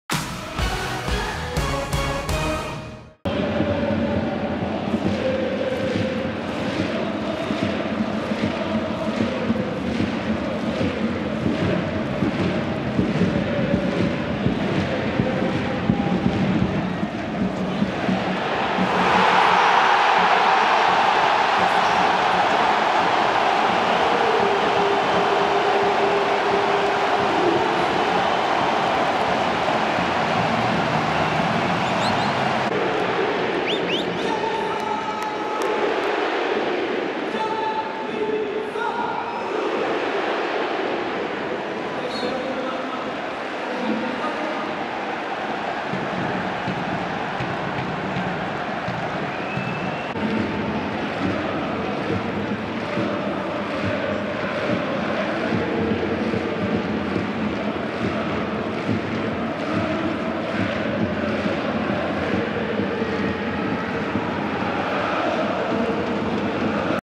Goal Kalifa COULIBALY (53' - FCN) FC NANTES - ANGERS SCO (1-1) 21/22
"FC NANTES - ANGERS SCO (1-1) Kalifa COULIBALY (53')  goal. FC NANTES vs ANGERS SCO Goals in video

Ligue 1 Uber Eats - Season 2021/2022 - Week 32 STADE DE LA BEAUJOIRE-LOUIS FONTENEAU - Sunday 17 April 2022